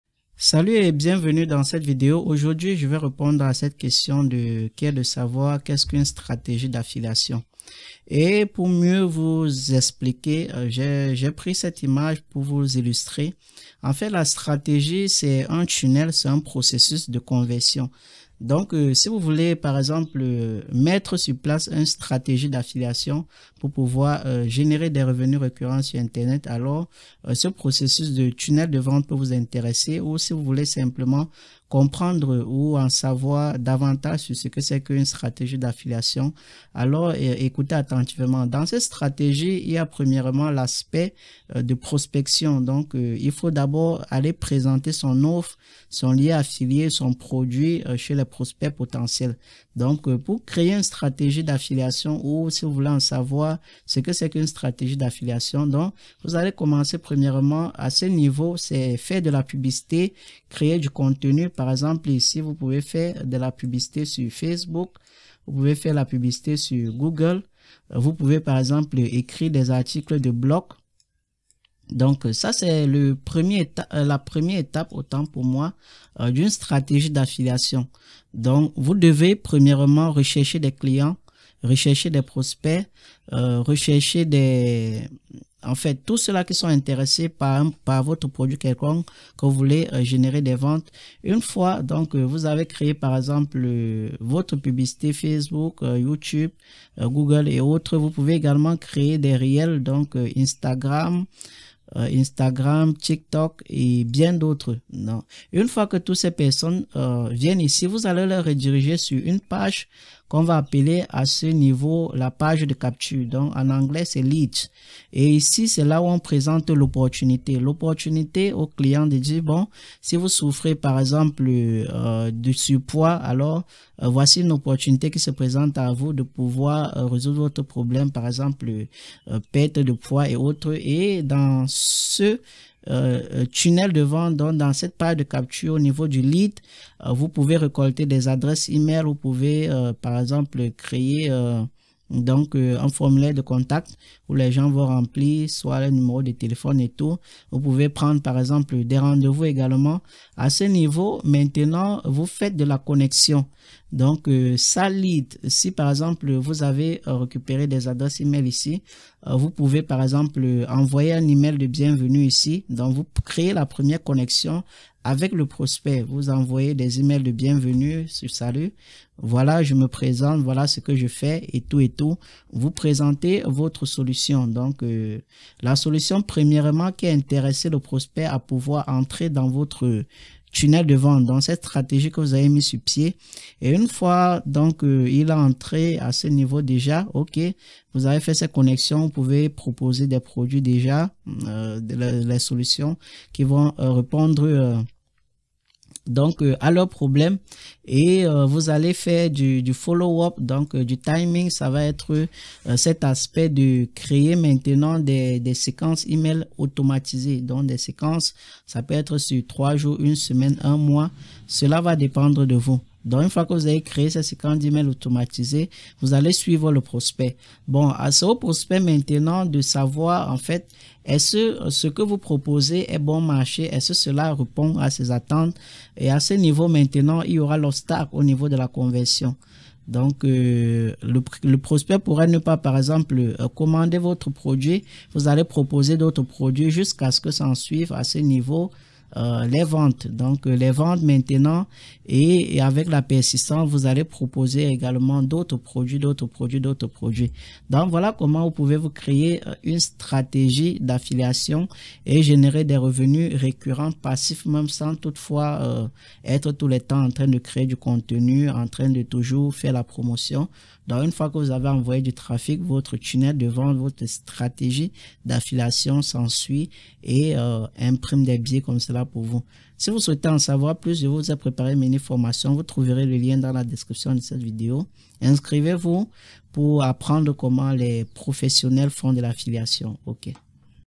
Qu'est-ce Qu'une Stratégie D'affiliation ⁉️ Démystifions l'Affiliation ✅
Dans cette vidéo captivante, plongez dans l'univers dynamique de l'affiliation en ligne ! Vous vous demandez ce qu'est réellement une stratégie d'affiliation et comment elle peut transformer votre présence en ligne en une machine à générer des revenus ?

Ne cherchez pas plus loin ! Nous décortiquons en profondeur les secrets d'une stratégie d'affiliation efficace. Découvrez les étapes clés pour choisir les bons produits, trouver les programmes d'affiliation appropriés et créer un contenu engageant qui incite à l'action.

Que vous soyez un créateur de contenu aspirant, un entrepreneur en ligne ou simplement curieux de savoir comment fonctionne l'affiliation, cette vidéo vous offre des conseils pratiques et des stratégies éprouvées pour réussir dans ce domaine compétitif.

Préparez-vous à franchir une nouvelle étape dans votre parcours en ligne et à découvrir comment une stratégie d'affiliation bien pensée peut ouvrir les portes de la réussite financière. Regardez dès maintenant et commencez à transformer votre passion en profit !